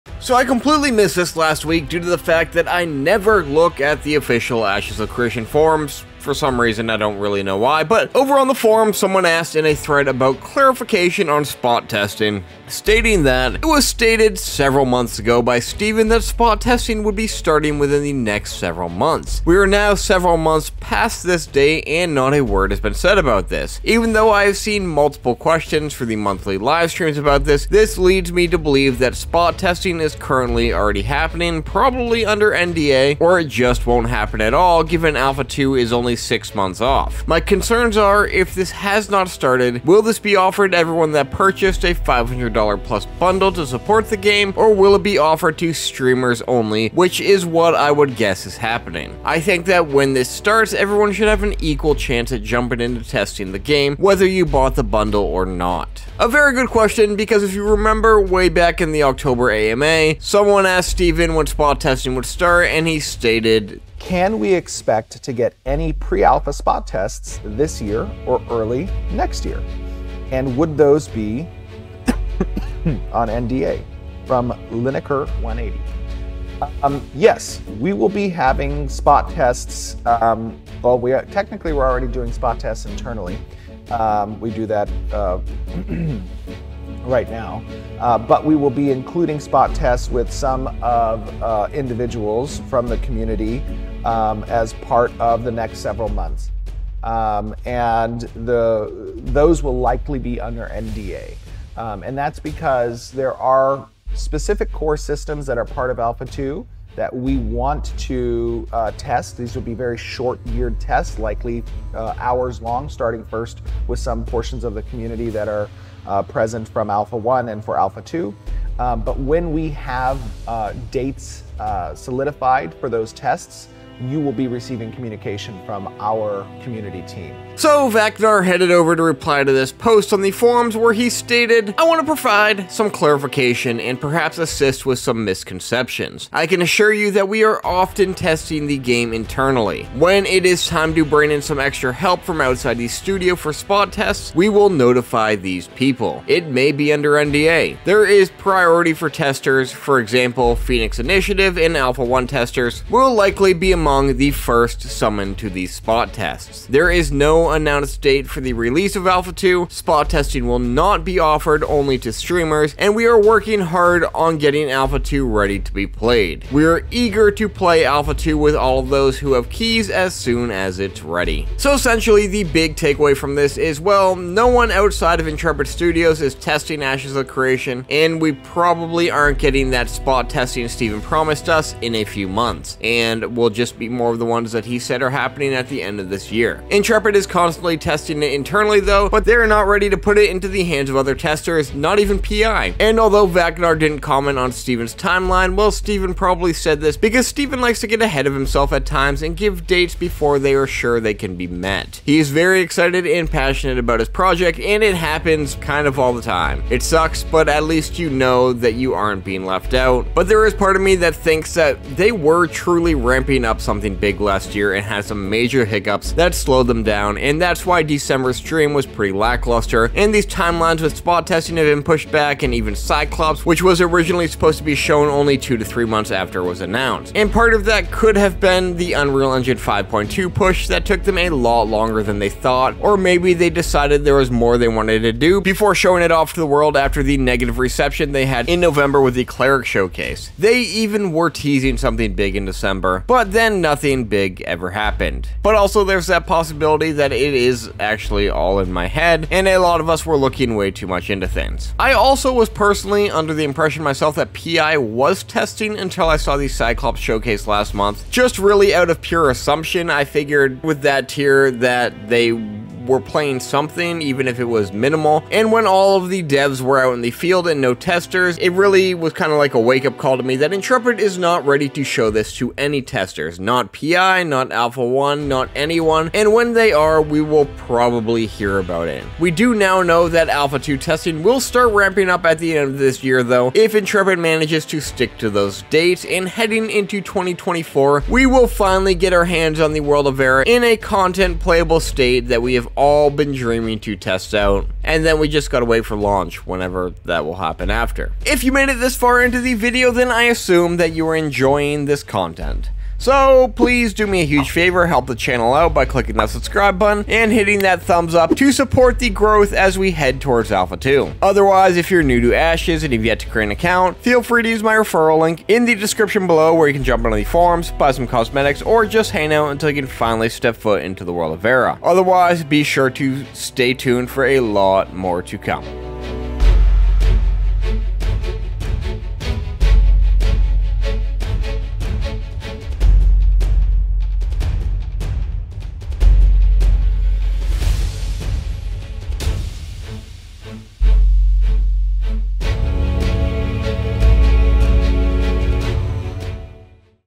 Ashes of Creation Gives an Alpha 2 Spot Testing Update
I'm not sure how I missed this post, but it is good to see Intrepid giving a bit more clarification on what is happening when it comes to Alpha 2 testing for Ashes of Creation!